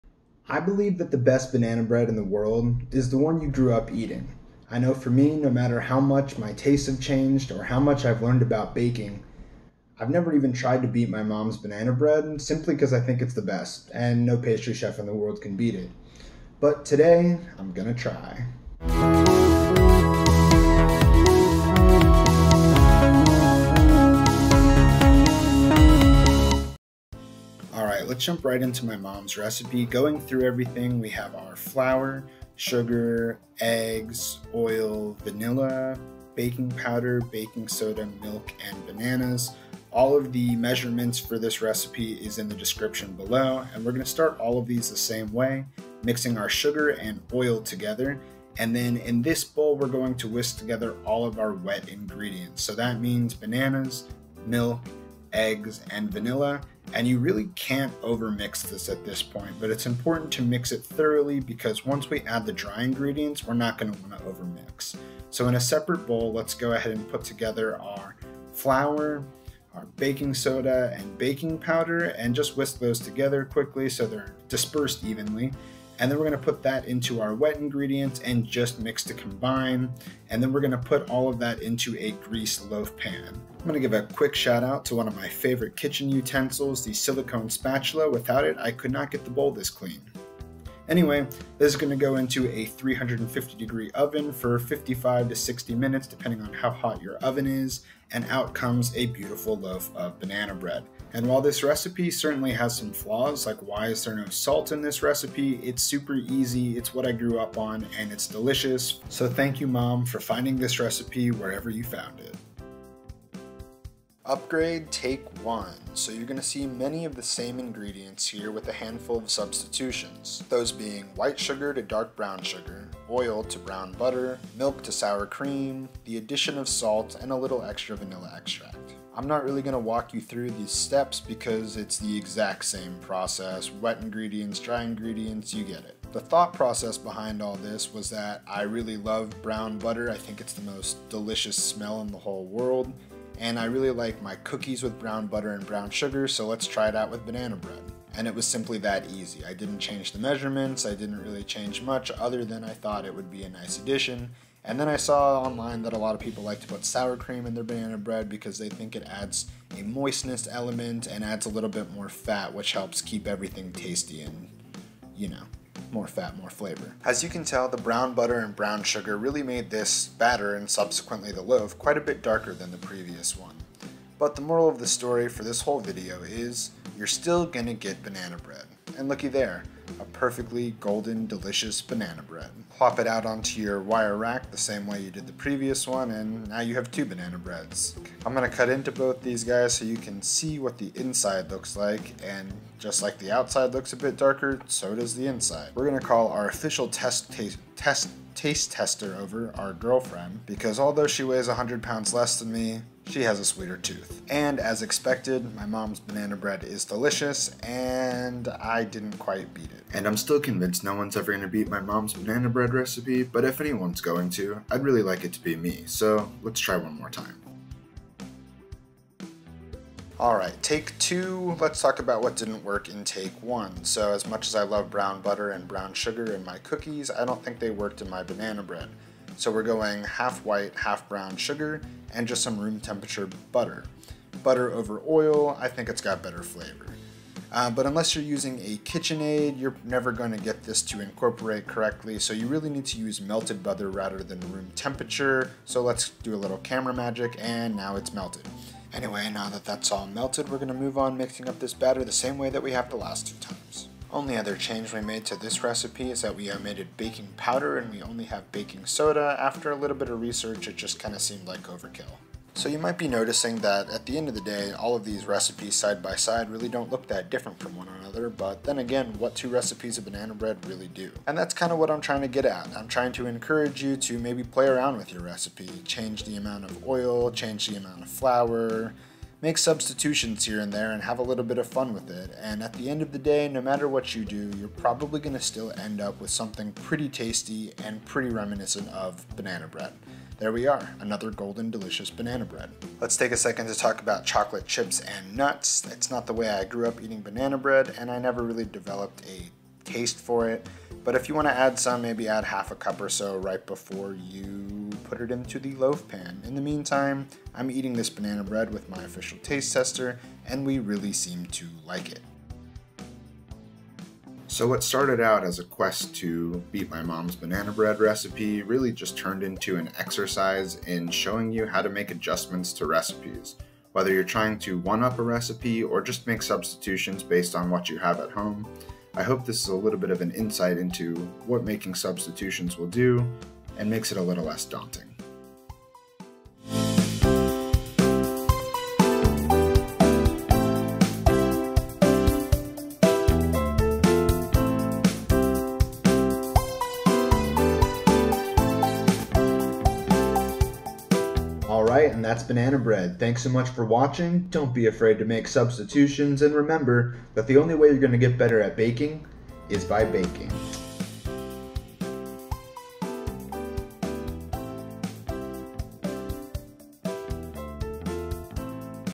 Making The Best Banana Bread
Banana bread is one of the most nostalgic foods for me, and I believe no banana bread is better than the one you grew up eating. This video shows you how to make substitutions to baking recipes in order to make the best banana bread possible for you.

These are good silicone spatulas
Bowl Scraper

Mom's Recipe
1/2c (112g) Oil (Any oil will do, I just used vegetable oil here)
1c (200g) White Sugar
2 Eggs
3 Bananas
2c (240g) All Purpose Flour
1 tsp (5g) Baking Soda
1/2 tsp (2g) Baking Powder
1/2 tsp (2g) Vanilla Extract
3 Tbsp (45g) Whole Milk
Bake @ 350 degrees F for 55-60 min

Upgrade Take 1
10 Tbsp (140g) Brown Butter (measure before browning, 1/2c or 112g after browning)
1c (200g) Dark Brown Sugar
2 Eggs
3 Bananas
2c (240g) All Purpose Flour
1 tsp (5g) Baking Soda
1/2 tsp (2g) Baking Powder
1 tsp (5g) Vanilla Extract
1/2 tsp (2g) Kosher Salt
1/3c (80g) Sour Cream

Upgrade Take 2
1 Stick (8 Tbsp/1/2c/112g) Melted Butter
1/2c (100g) Dark Brown Sugar
1/2c (100g) White Sugar
2 Eggs
3 Bananas
2c (240g) All Purpose Flour
1 tsp (5g) Baking Soda
1 tsp (5g) Vanilla Extract
1/2 tsp (2g) Kosher Salt
1/3c (80g) Sour Cream
Bake @ 350 degrees F for 55-65 min